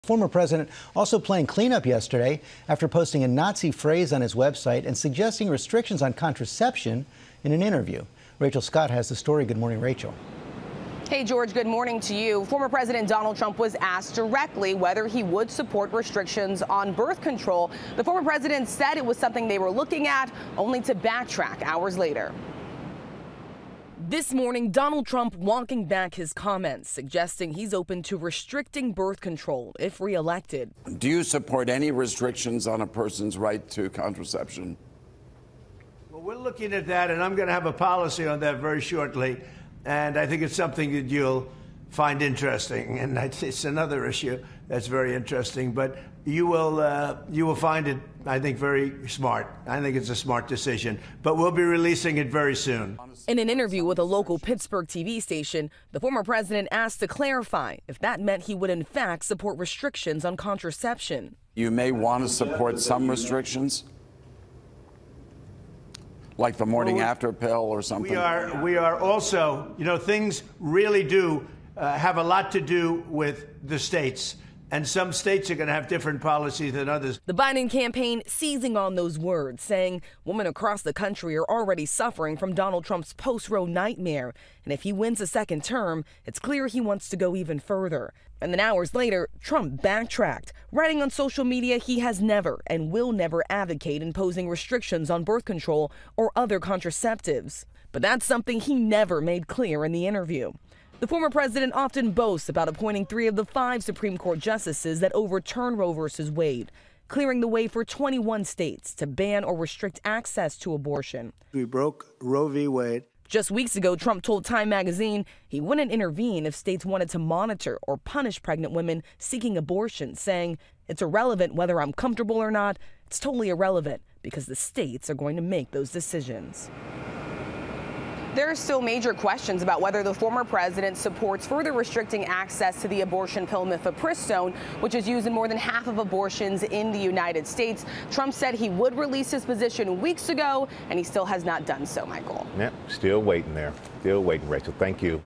Trump tries to clean up comments about restricting contraception
Former President Donald Trump is scrambling to clean up comments suggesting he is open to restricting birth control if reelected.